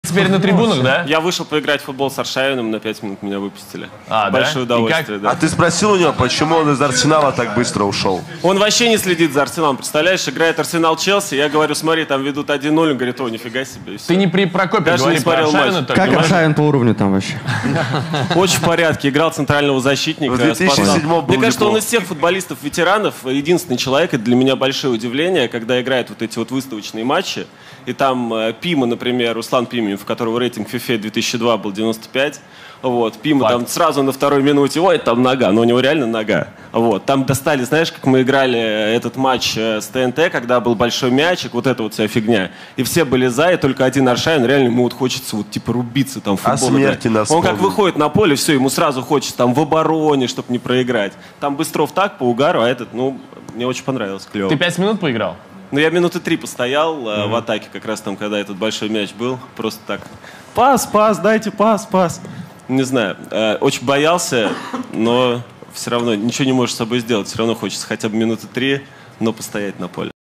КАК ДИМА ЕГОРОВ С АРШАВИНЫМ В ФУТБОЛ ПОИГРАЛ
Канал: @broke_boys_official